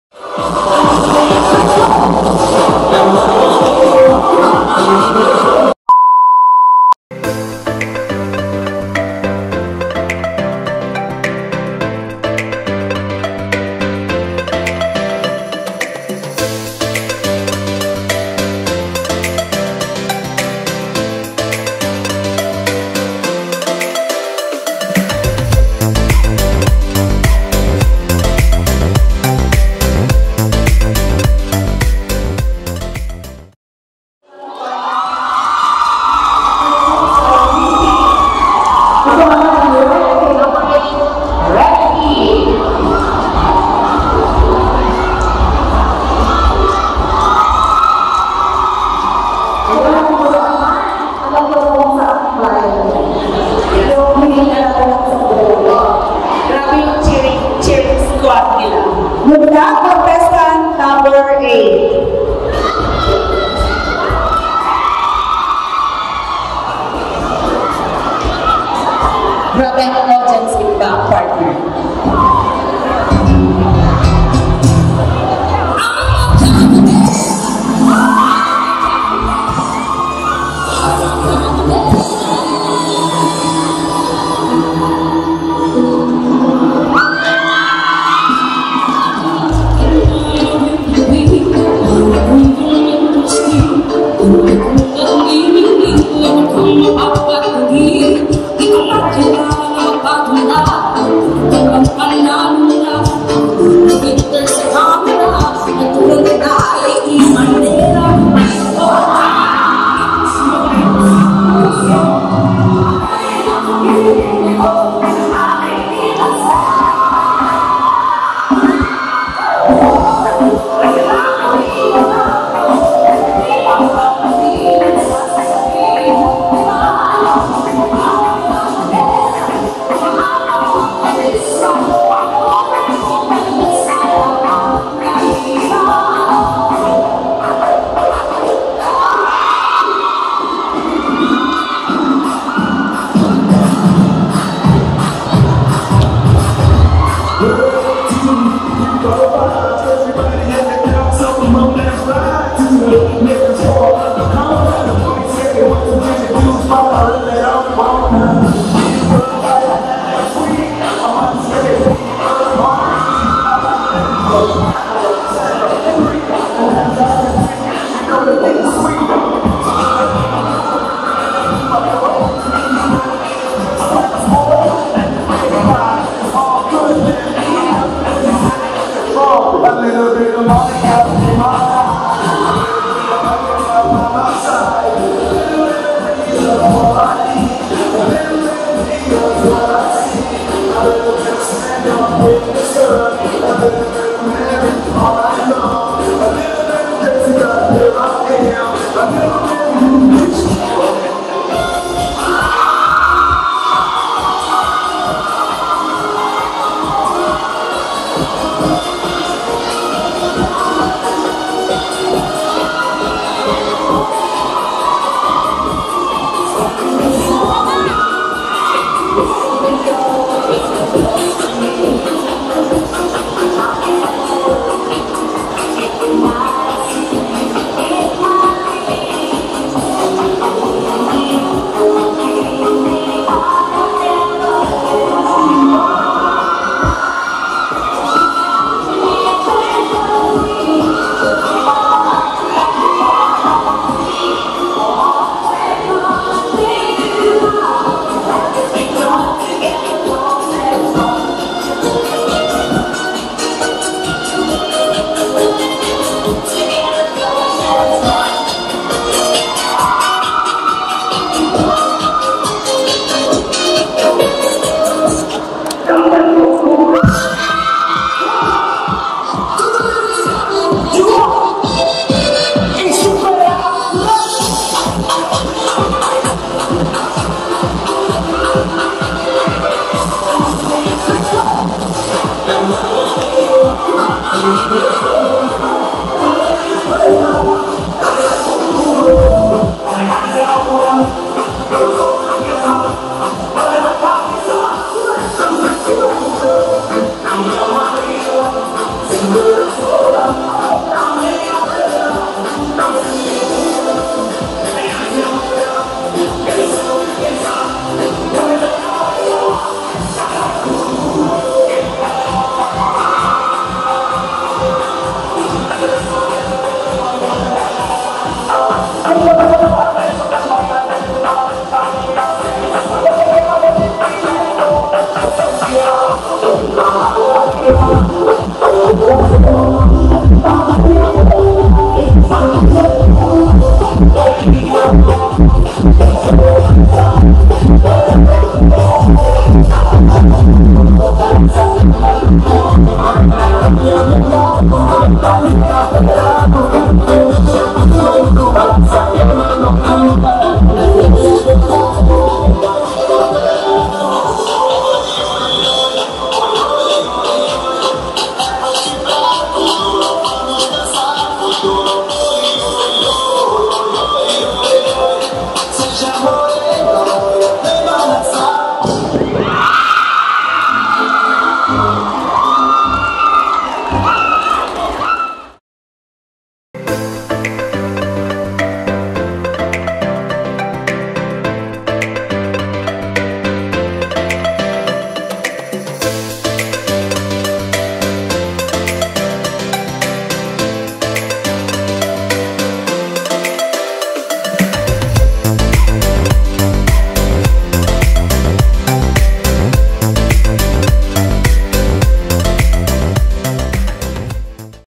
RIC Women Achievement Day 2022 | mapapabilib ka sa paghataw
grand champion for the ballroom dance competition.